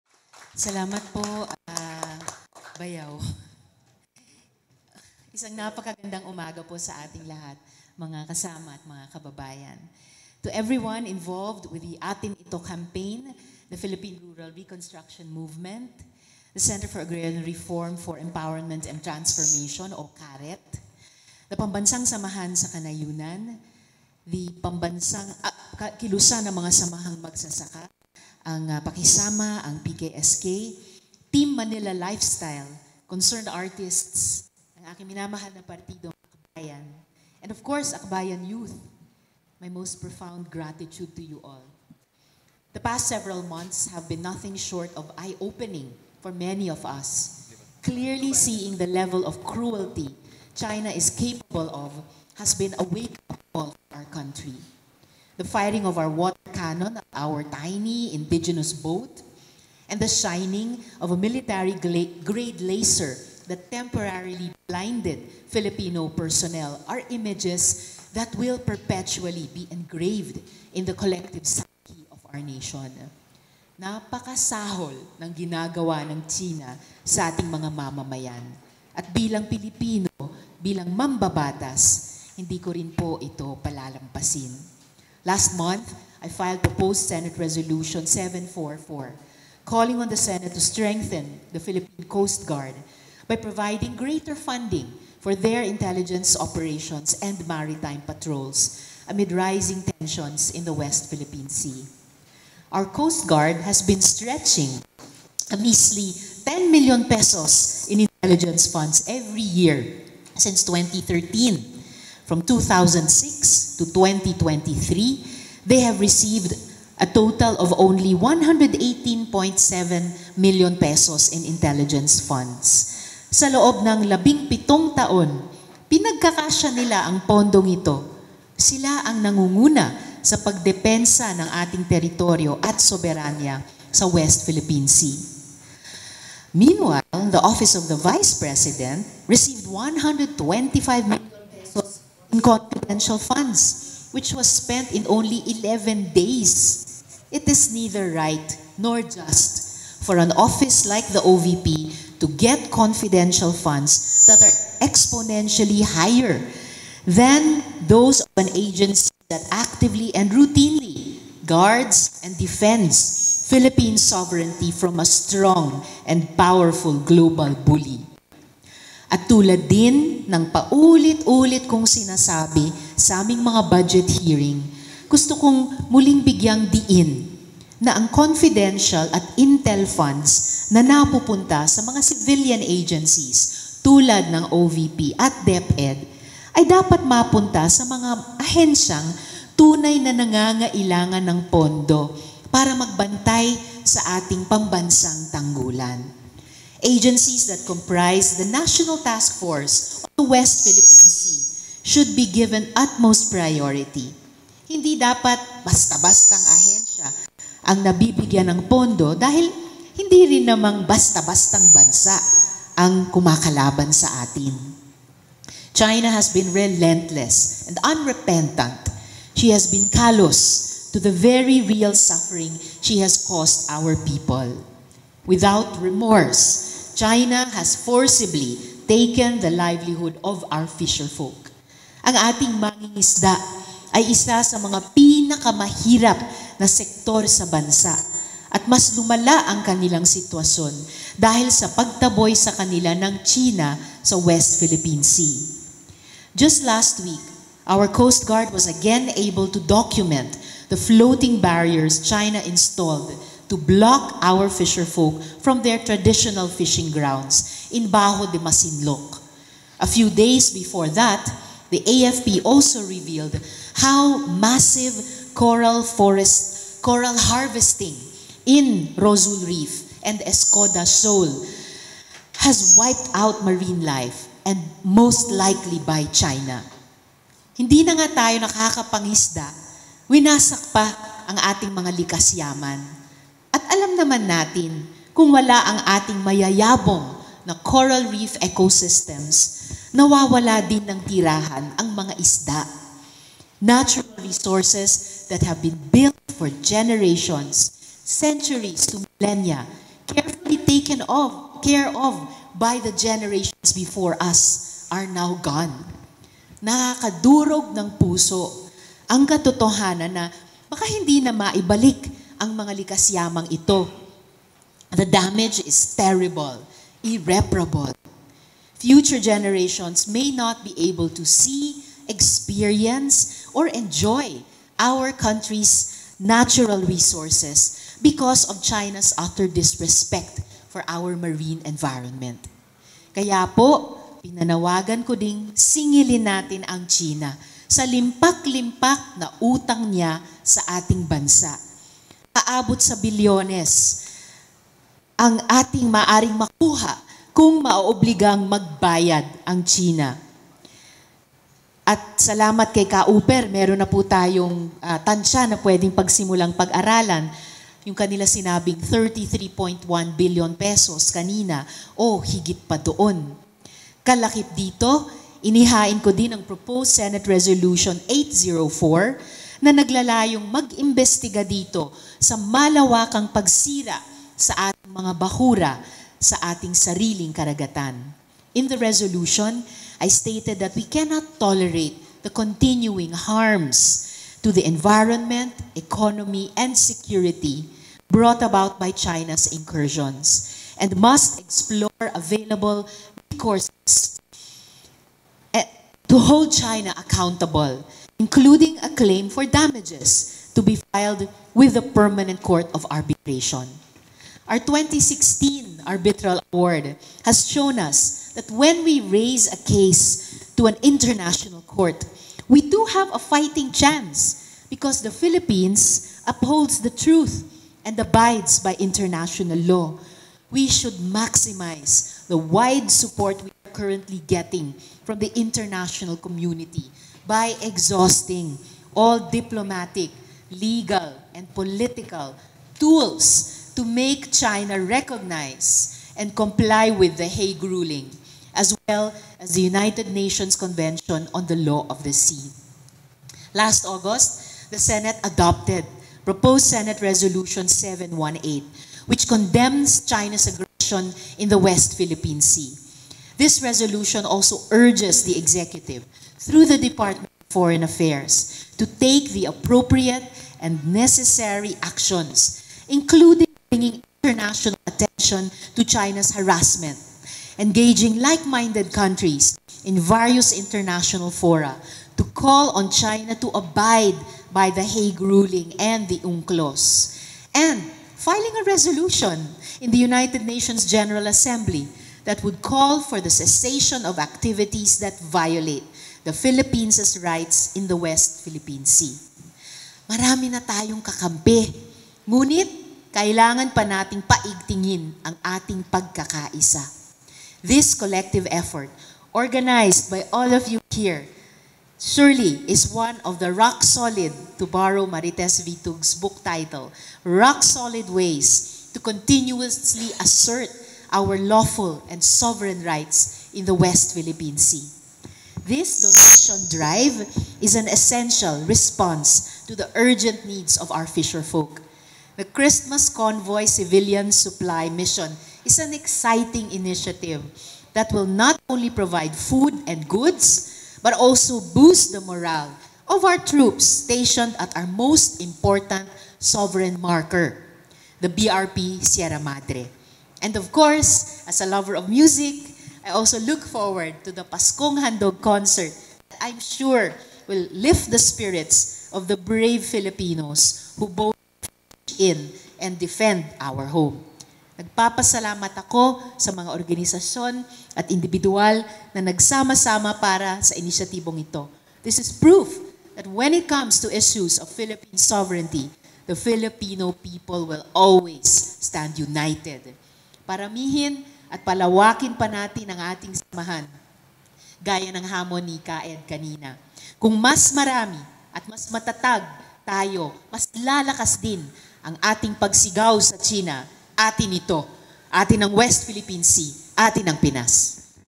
Senator Hontiveros: ‘Singilin natin ang China sa limpak-limpak na utang niya sa ating bansa’
At a press conference for the launch of “Atin Ito!” the minority senator says it’s high time the Philippines make China answer and pay for the damage their ships have caused in the West Philippine Sea. She also bemoans the measly intelligence funds the Philippine Coast Guard gets in contrast to Vice President Sara Duterte’s Office of the Vice President and even the Education department.